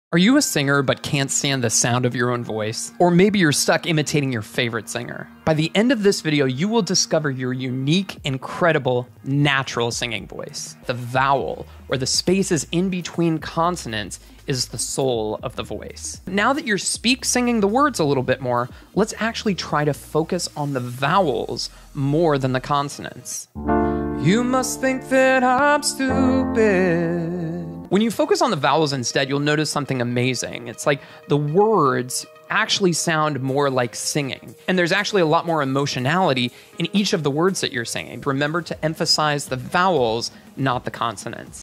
HOW TO SING with Your NATURAL Voice Pt 2
Every singer needs to find their natural singing voice.

But finding your true, unique voice can be really difficult.

Today, I'll show you my 3-step process for finding your true voice.

This is Part 2 of a series.